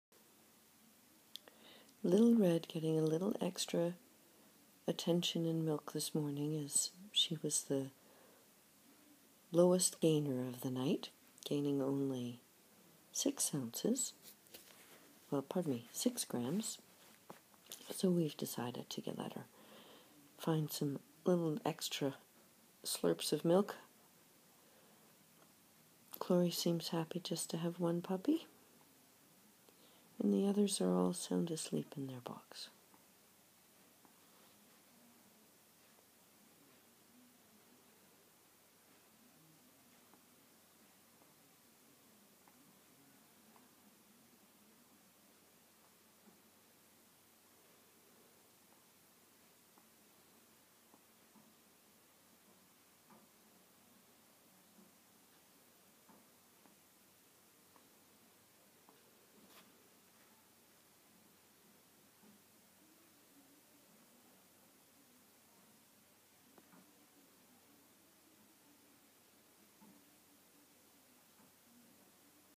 Glory takes time with little red collar, Sidney
Tidewalker Coastal Sidney gets a little special time with Mom, Glory.